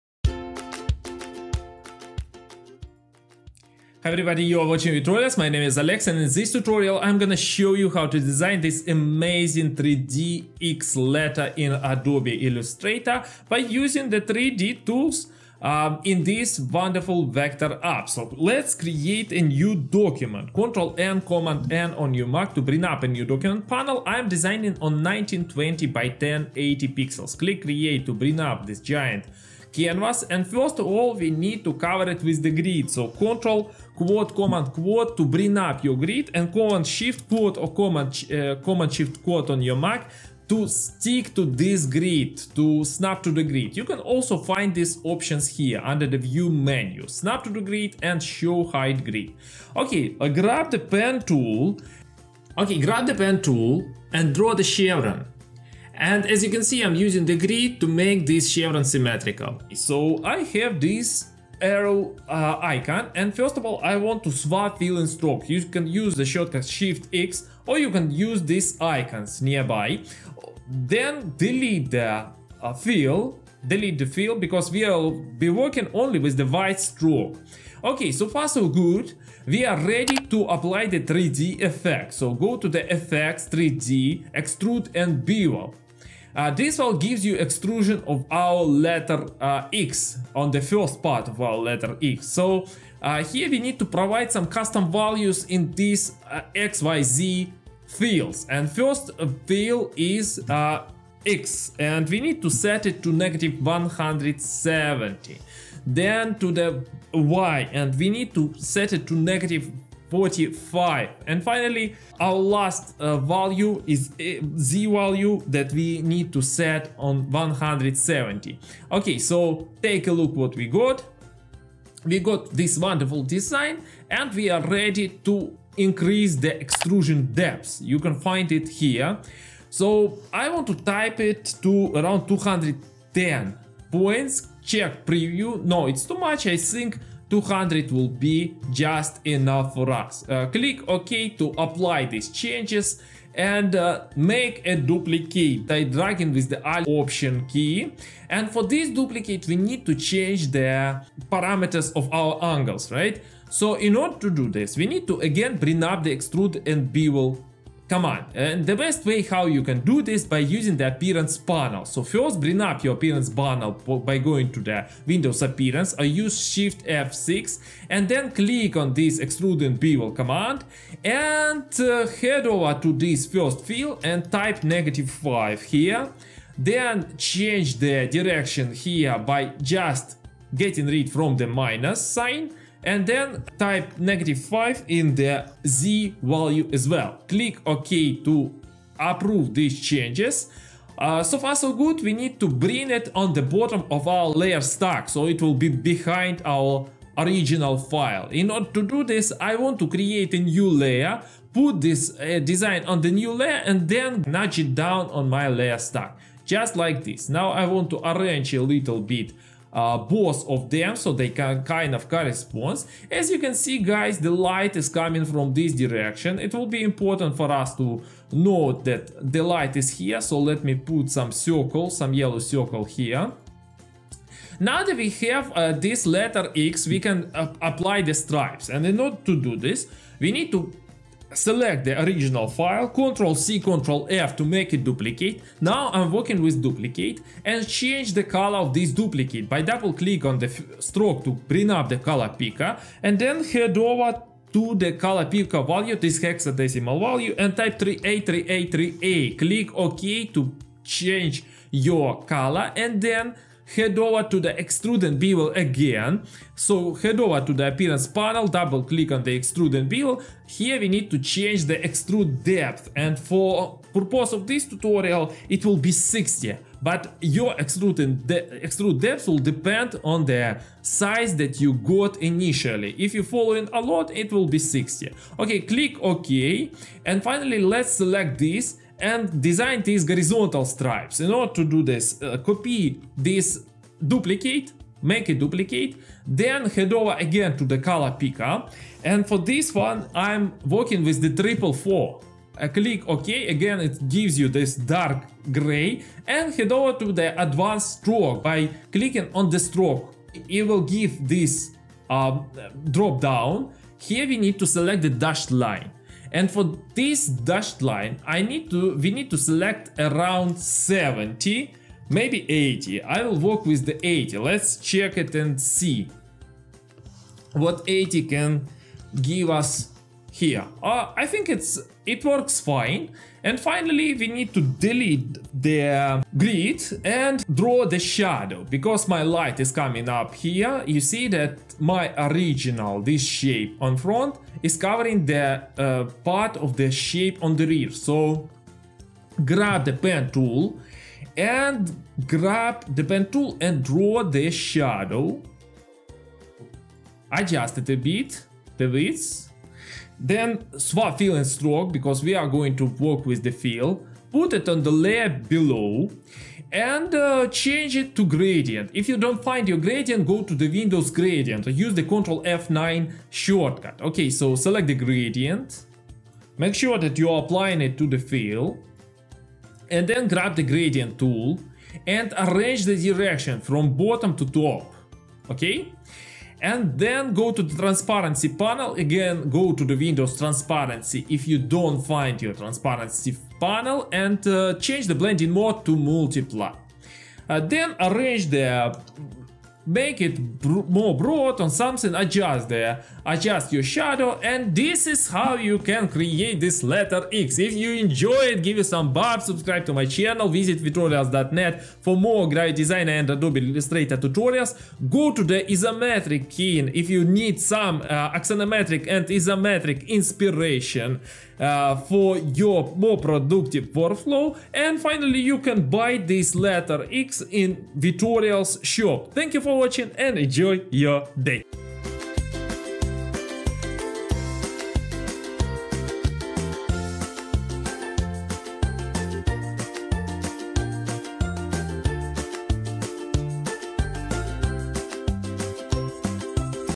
Flat 3d logo design in Illustrator. How to design a minimalist letter logo in Adobe Illustrator
In today's tutorial, we are going to design a minimalist 3d logo in Adobe Illustrator.

First, you will learn how to get a 2d letter mark with the pen tool. Then I will show you how to leverage the 3d tool to put this logo into the axonometric plain.
After we will get additional design elements (black lines and stripes) by modifying the duplicates.

Timestamp:
1. Intro:
2. Designing a letter X
3. Designing 3d letter using 3d/extrude and bevel:
4. Working with duplicates:
5. Adding shadows for 3d look: